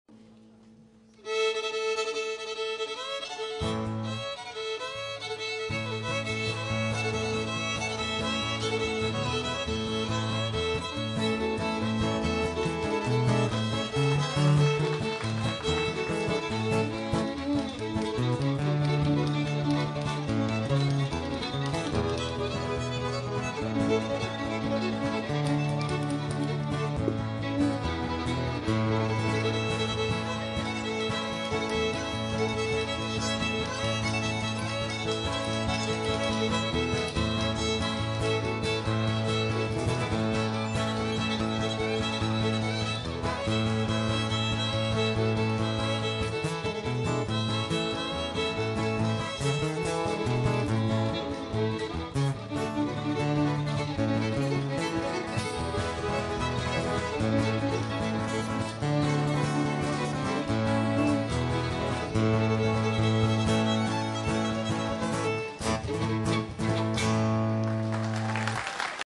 11 year old Amelia Eisenhauer wins Instrumental Division
11 year old Amelia Eisenhauer from DuQuoin,Il, was the winner in the Instrumental Division of the Kentucky Opry Talent Search on Feb 26,2011. She will move into the Semi-Finals round of competition on May 7 2011.